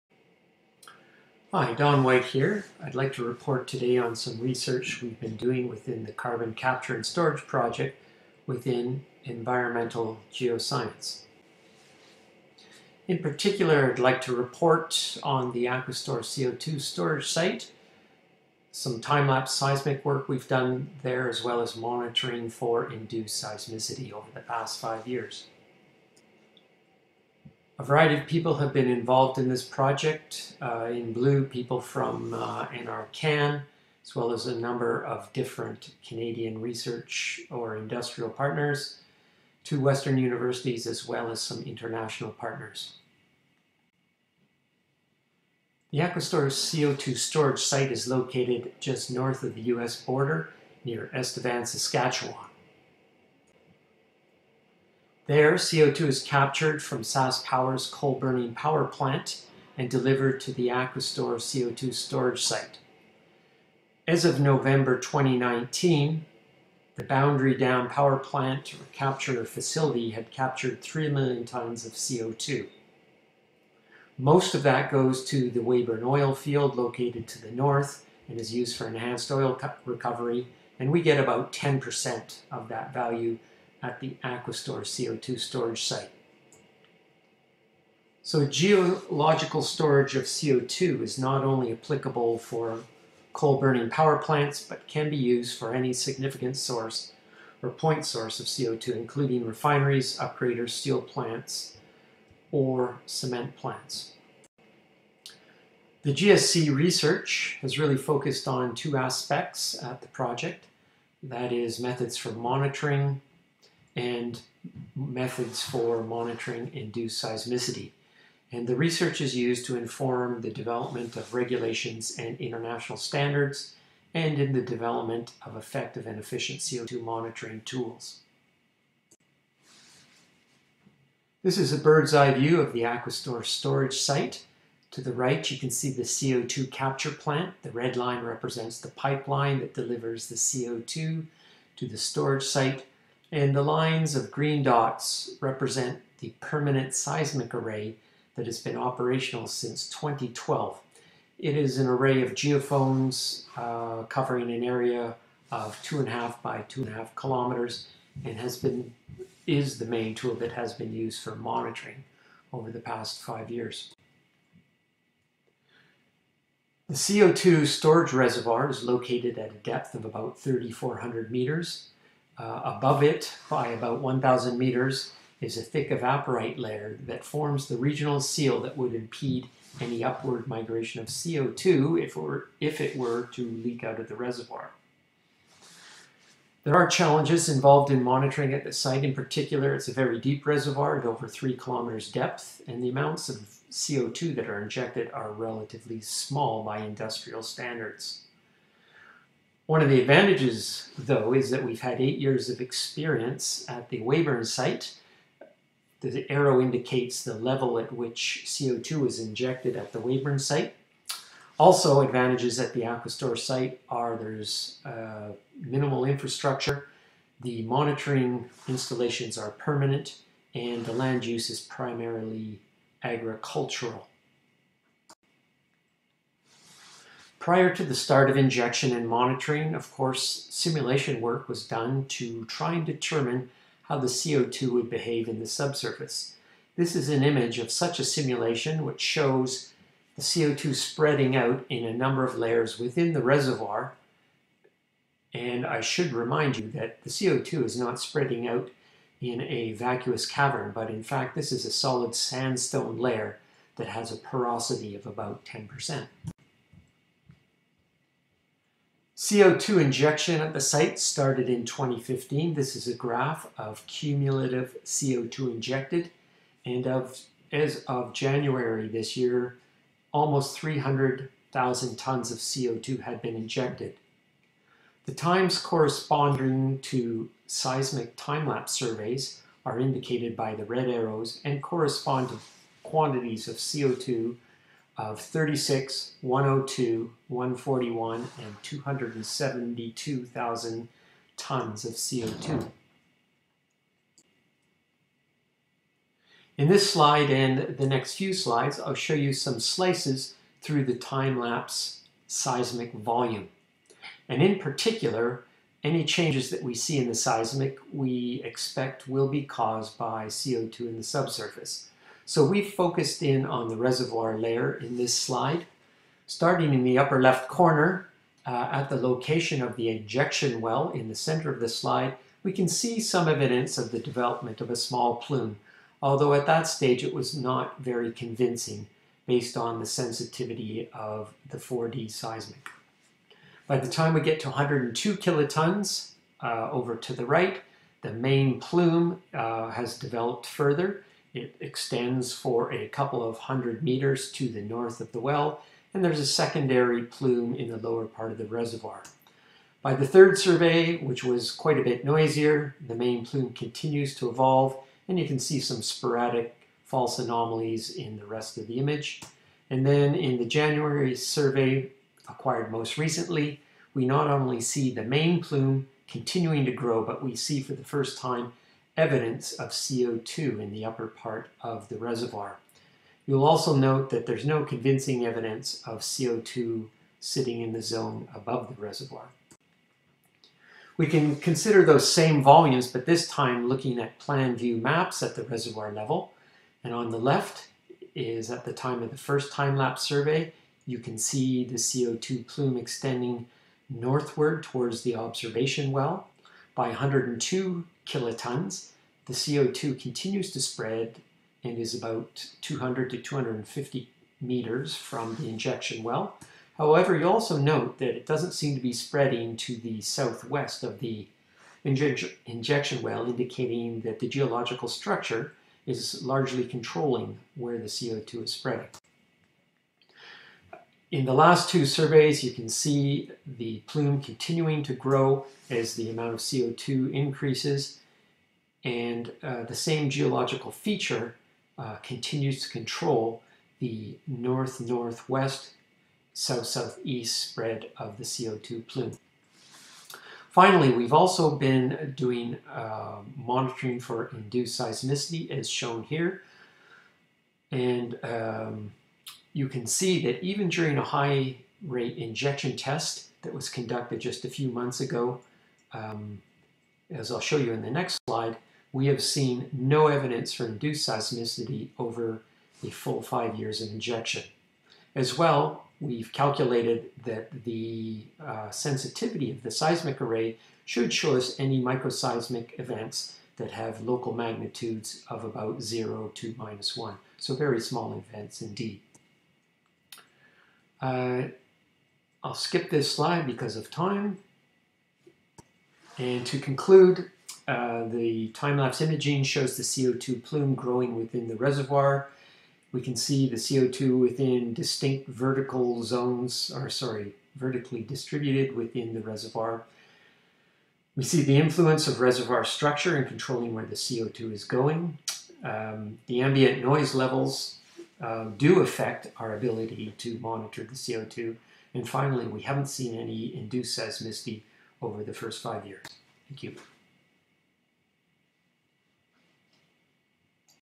Measuring, monitoring and verifying geological carbon storage, Dr. Don White
Aquistore CO2 storage: 5 years of monitoring

- Time lapse seismic
- Induced seismicity